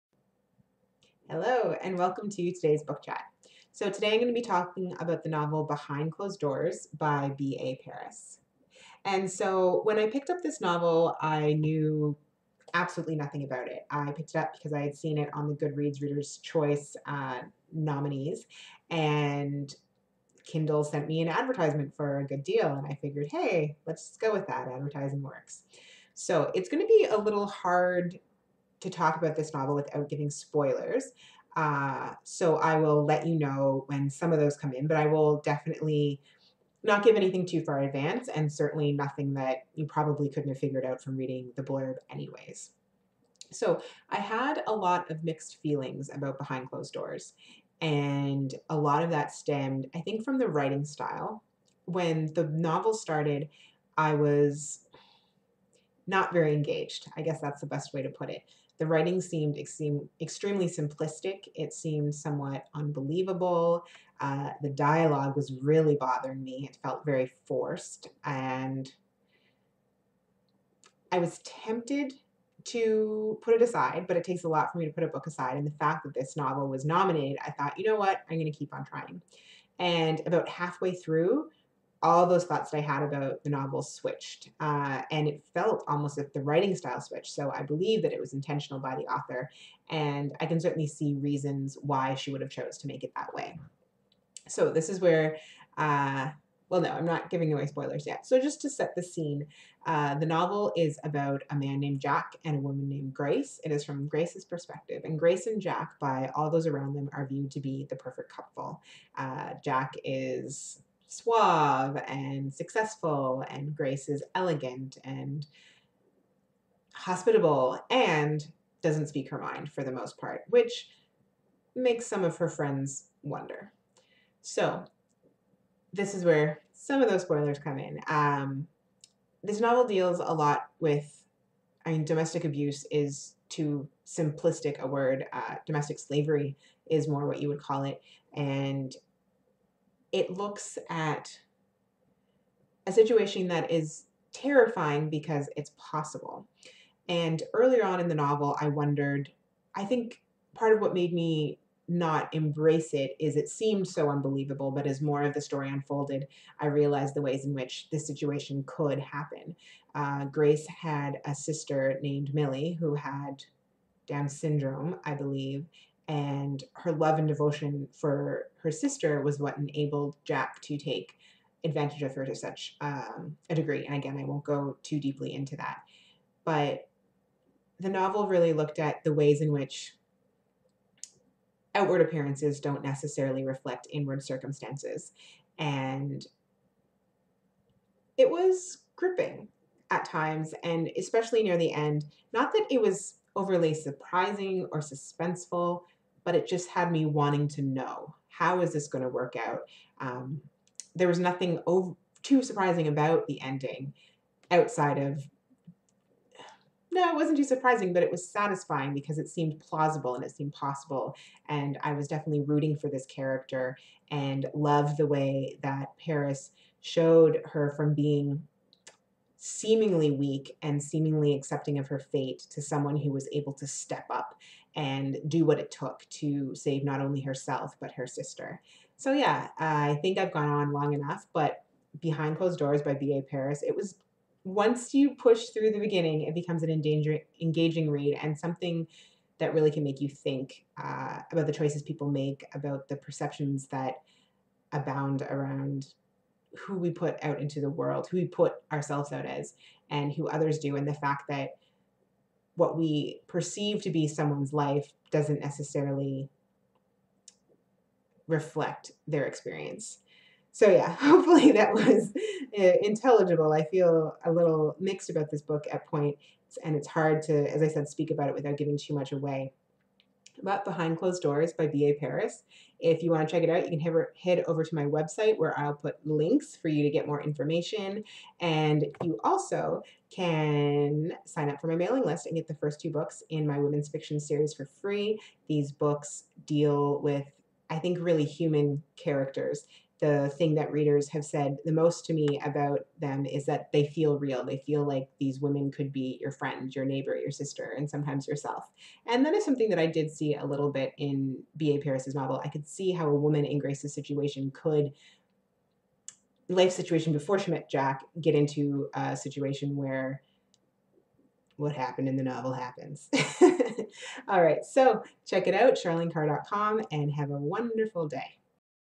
Book Chat: Behind Closed Doors by B.A. Paris
A pretty slow and unsatisfying start, but it redeems itself, turning into a book full of reflection with a nice dose of uncertainty and suspense.